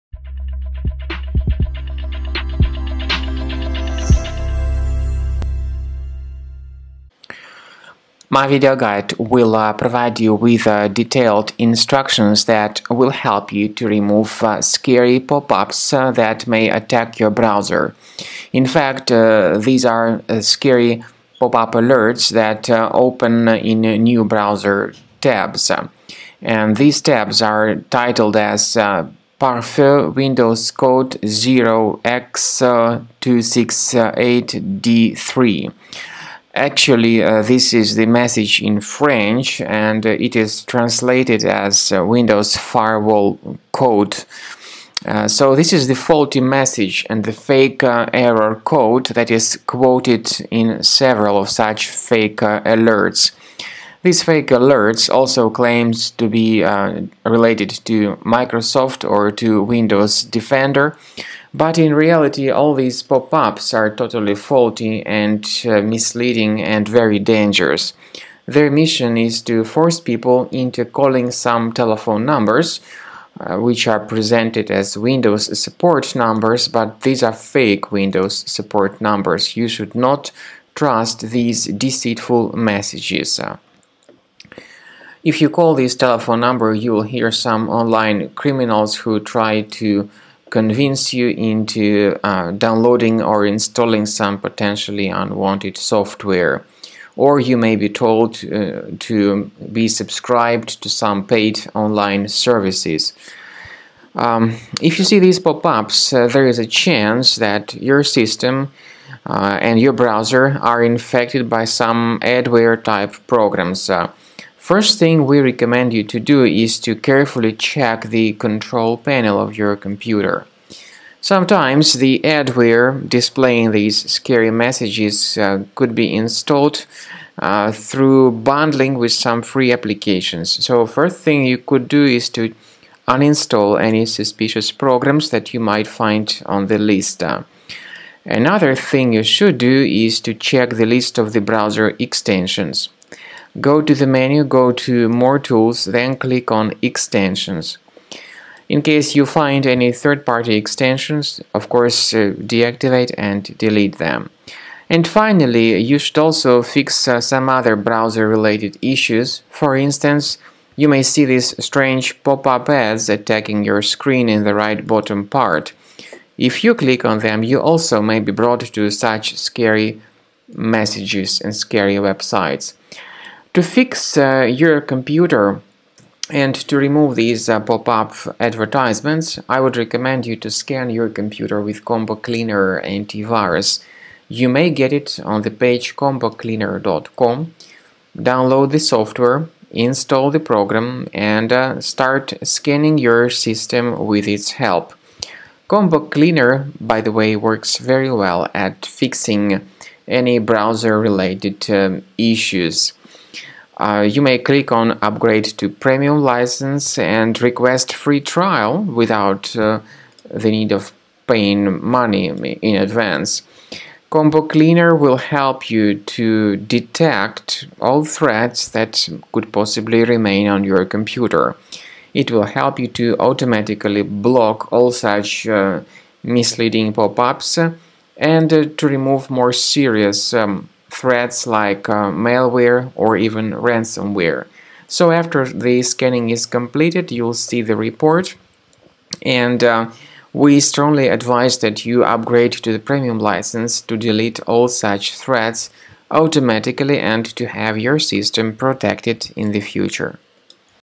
Supprimer Pare-feu Windows Code0x268d3 arnaque.
Pare-feu Windows Code0x268d3 is a scary alert that now keeps attacking many Windows computers and all available browsers. The alert claims to be related to Windows Firewall, however, it is a typical scam. Remove it with Combo Cleaner Antivirus or follow the manual guidelines set forth in the video.